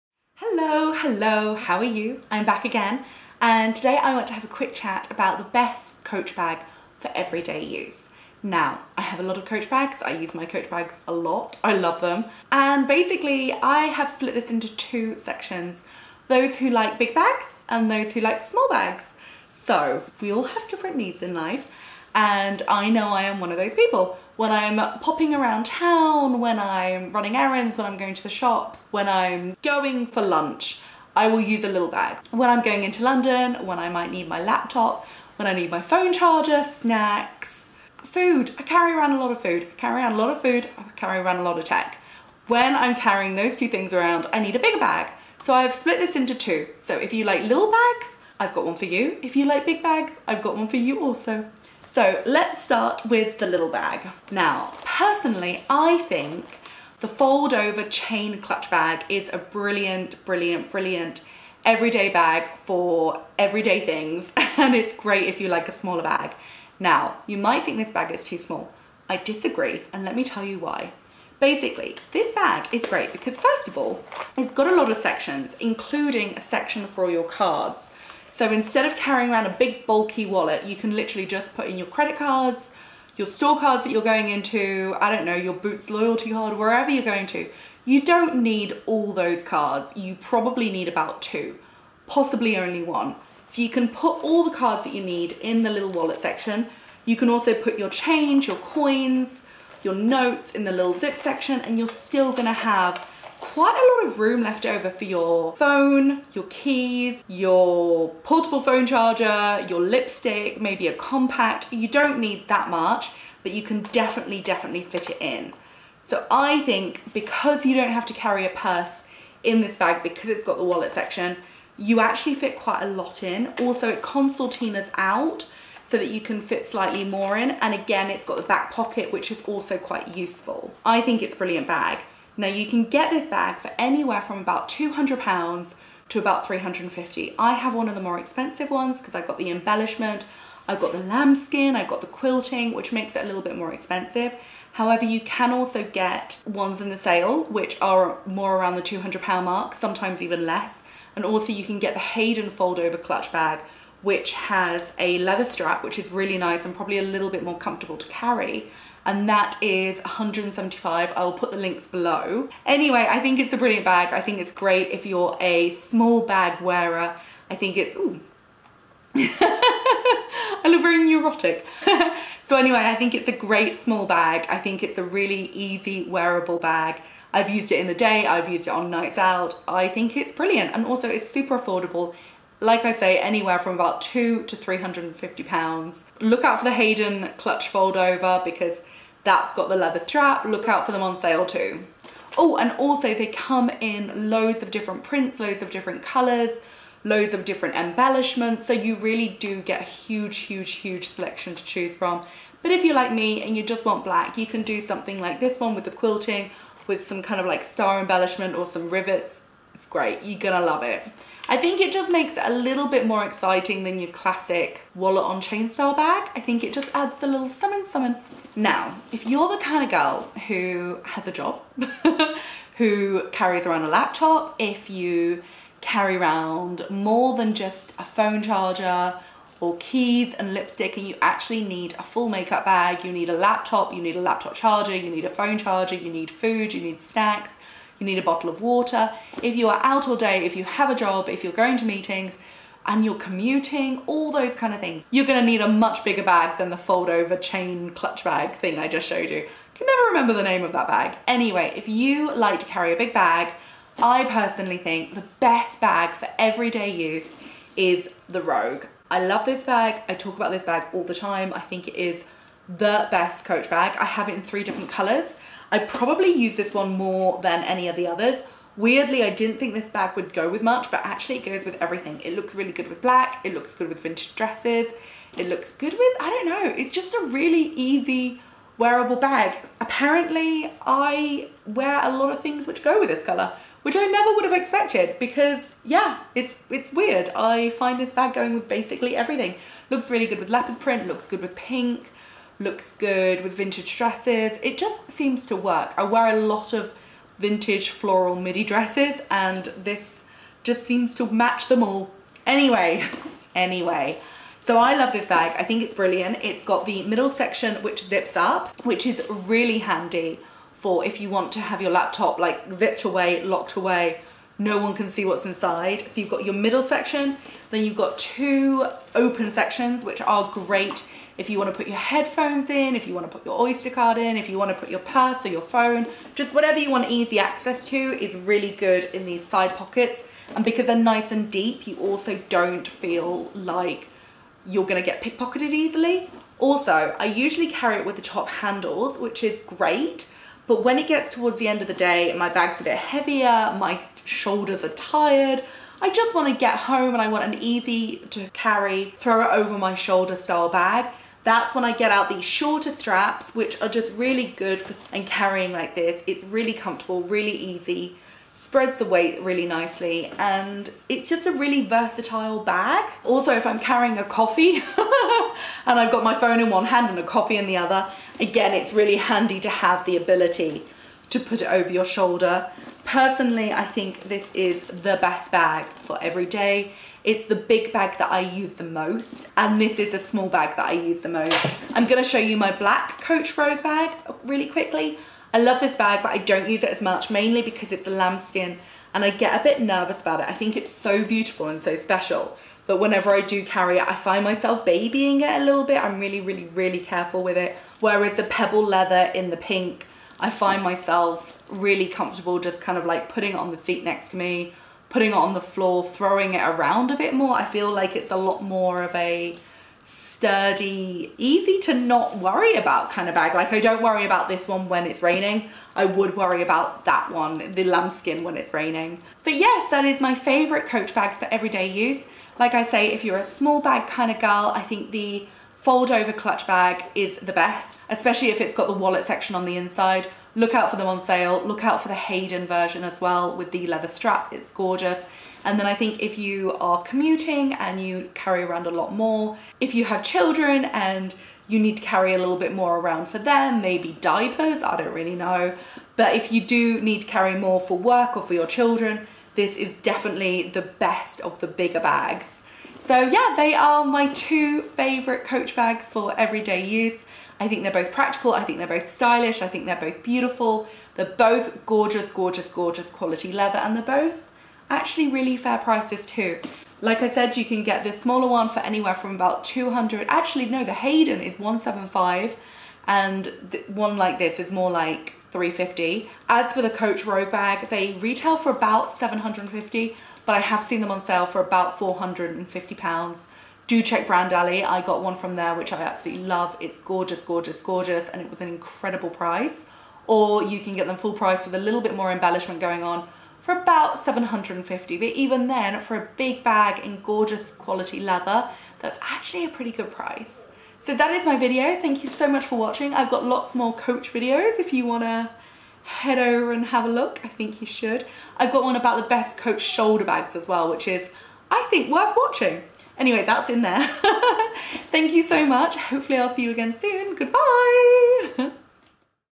Best Coach Bag For Every Day Use // Coach Handbag Review
Hello friends! I'm back again with another coach video about the best Coach  bags for every day use! As you might already know, I LOVE coach and I have a nice little collection of them, but today I want to show you my two best every day coach bags, one for big bag ladies, one for smaller bag ladies! From the Rogue to the Riley, the Parker to the Callie, and of course, how could I forget the canteen bag too, there are so many to discuss!

My 'Dream Big' Sign is from Etsy

Video Filmed using the Olympus Pen Camera

With the Olympus 12mm Lens

xxxxxxxxxx